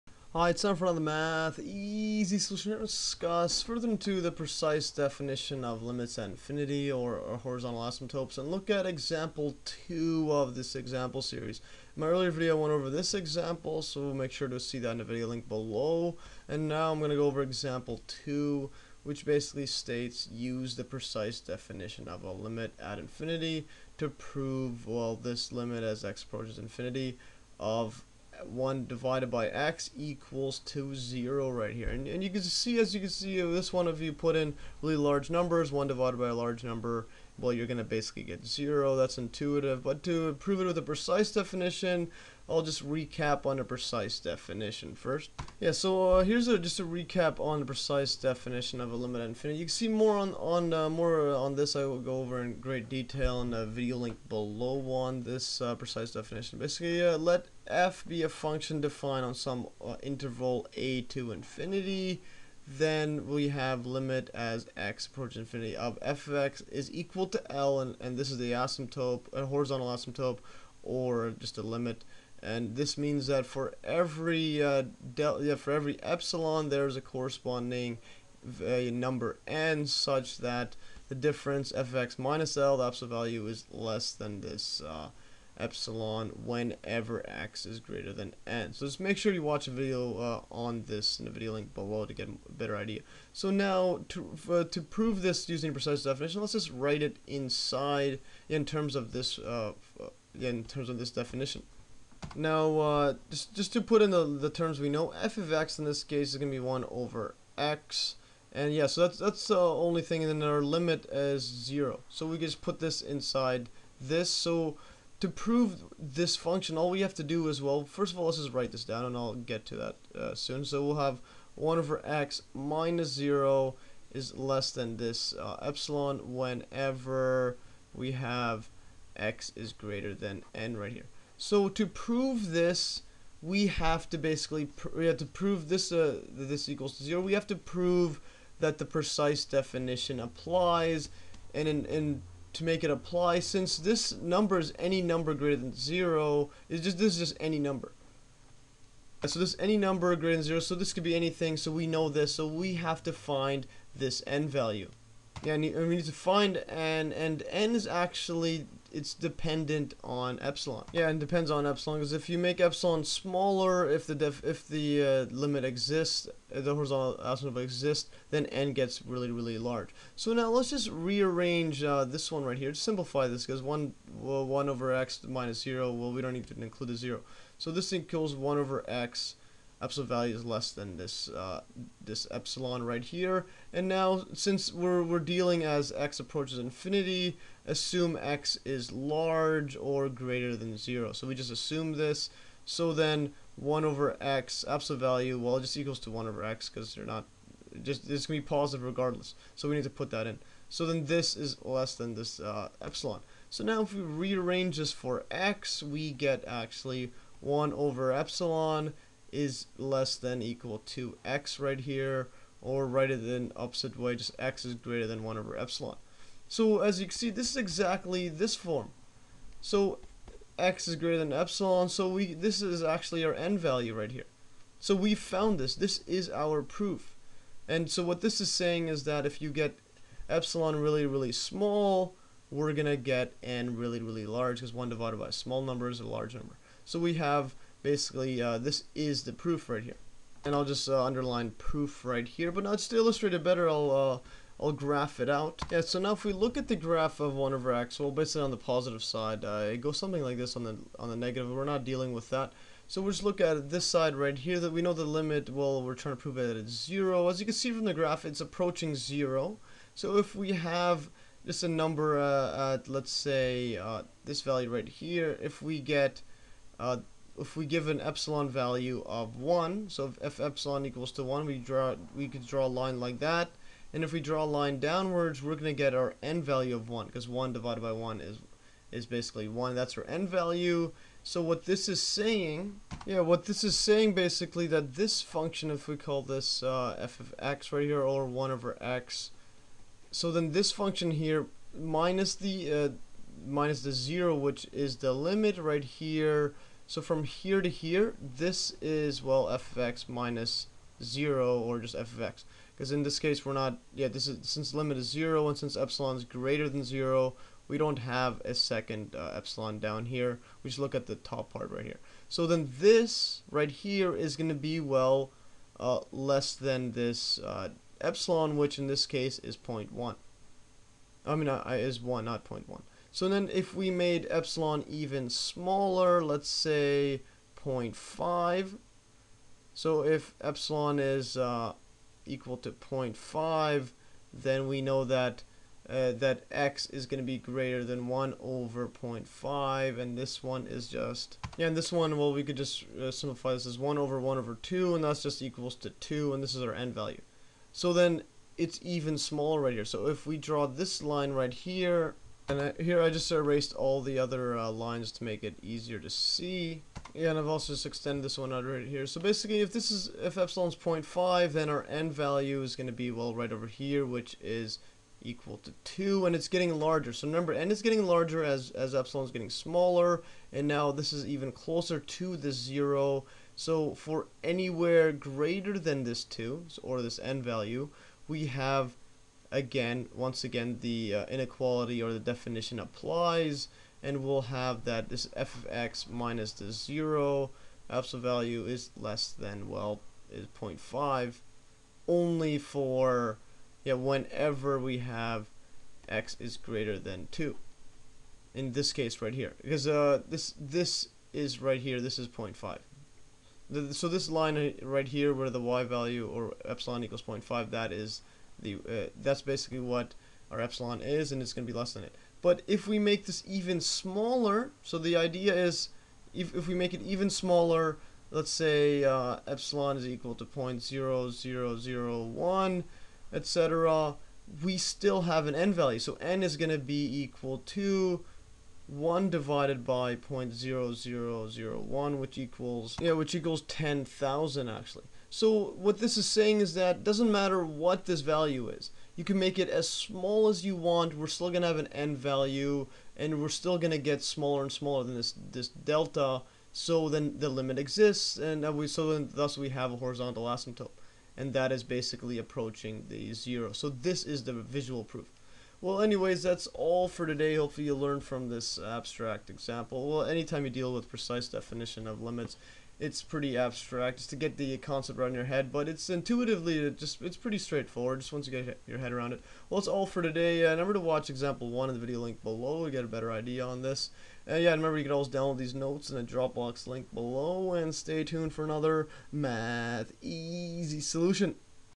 Precise Definition of Limits at infinity: Example 2
In this video I go over a second example on using the precise definition of limits at infinity to prove that the limit as x approaches infinity of the function 1/x equals 0.